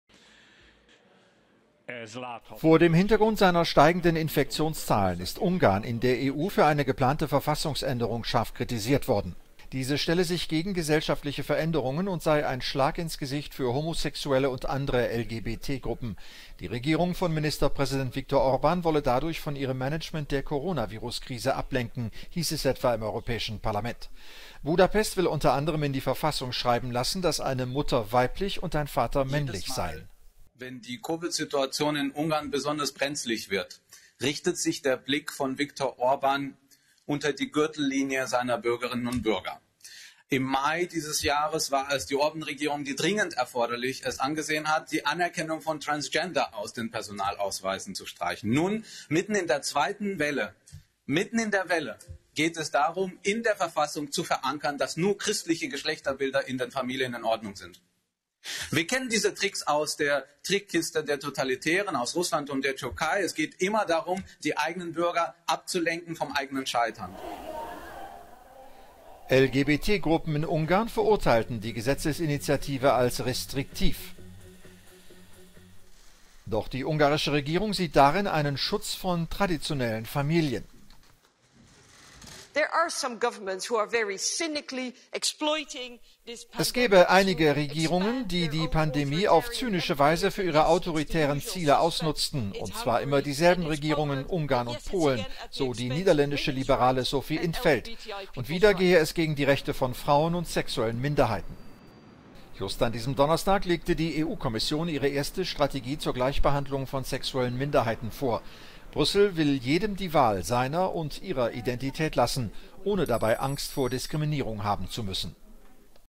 Scharfe Kritik an Orbans LGBT-Politik
Ungarns MInisterpräsident wolle mit seiner restriktiven Politik nur vom eigenen Versagen in der Coronavirus-Krise ablenken, heisst es.